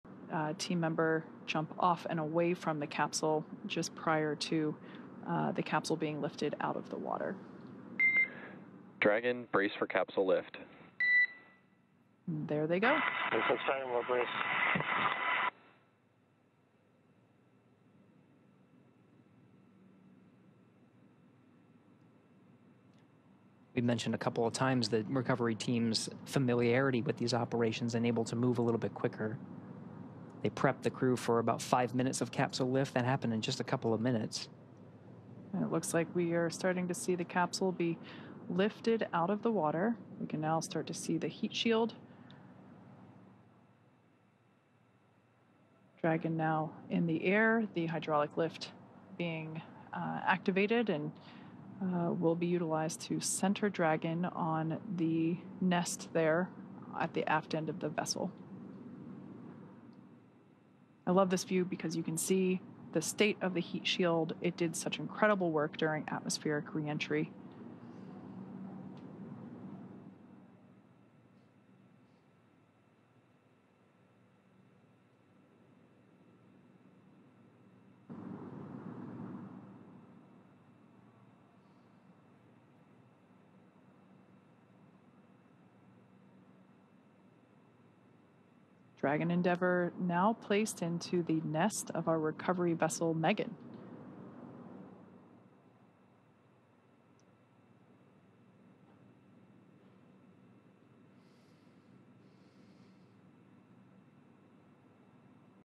SpaceX capsule with crew recovered from ocean
Atlantic Ocean - 4 September 2023
1. Member of ground crew jumps off capsule into ocean
2. Capsule is lifted out of water and onto ship

STORYLINE:
Four astronauts returned to Earth early Monday after a six-month stay at the International Space Station.

Their SpaceX capsule parachuted into the Atlantic off the Florida coast.

Less than an hour later, the capsule was lifted out of the ocean and onto a ship.

Returning were NASA astronauts Stephen Bowen and Warren “Woody” Hoburg, Russia's Andrei Fedyaev and the United Arab Emirates' Sultan al-Neyadi, the first person from the Arab world to spend an extended time in orbit.